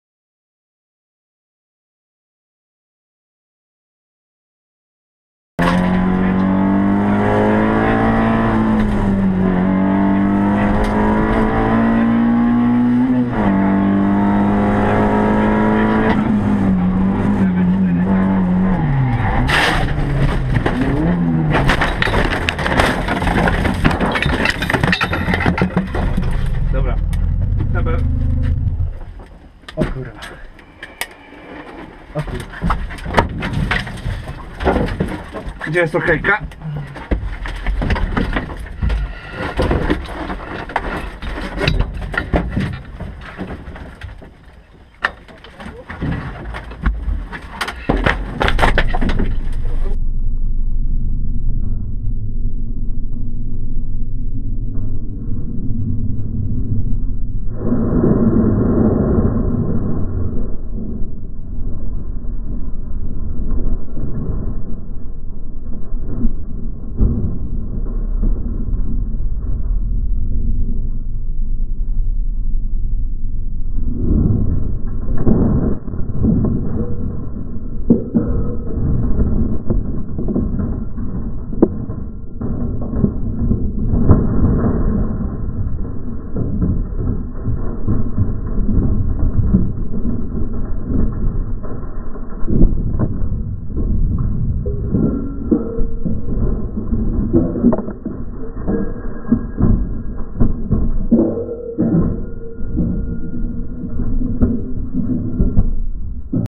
Rajd Rzeszowiak 2015 OS 2 Smolinski Kędzierski Citroen C2 NIEBO
Rajd Rzeszowiak 2015 OS 2 Smoliński / Kędzierski Citroen C2 Crash